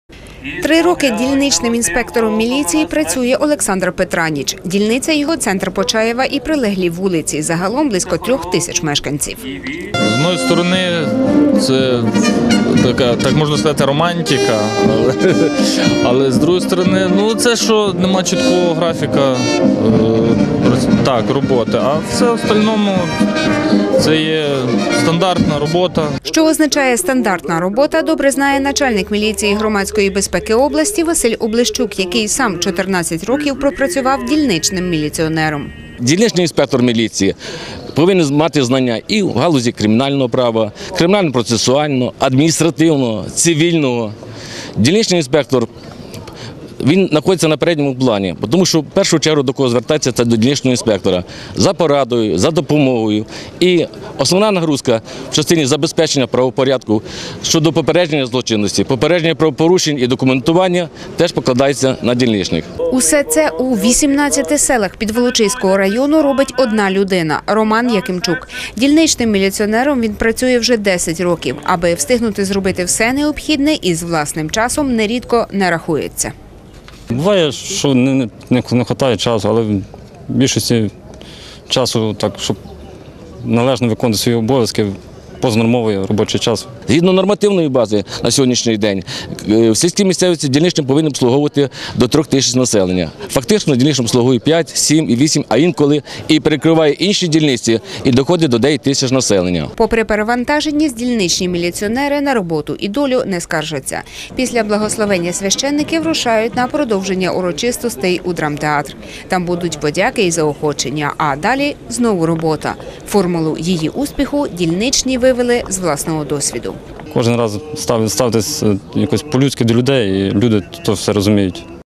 18 червня - 90 років службі дільничних міліціонерів
Чергові звання, подяки, заоохочення. Дев'яносту річницю завтра відзначить служба дільничних інспекторів міліції. 18 червня 1923 року був виданий перший нормативний документ - Інструкція, який юридично утвердив цю службу. На Тернопіллі нині працює 343 дільничних міліціонери, 27 з яких - жінки. Робота з людьми і не завжди чесними - тому й нелегка. Відтак дільничних інспекторів на Тернопіллі бракує.